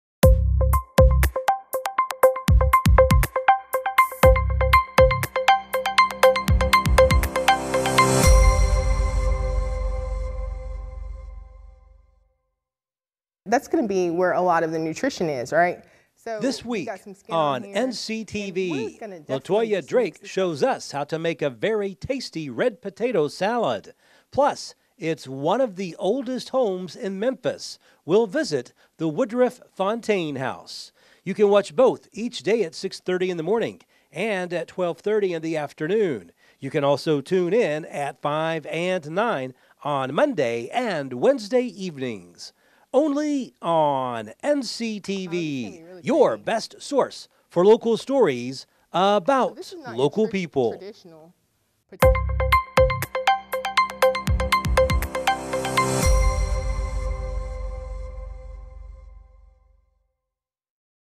Recipe - Historic Memphis Mansion Promo
This week on NCTV, we learn how to make a delicious red potato salad, plus, we visit a mansion filled with history in Memphis!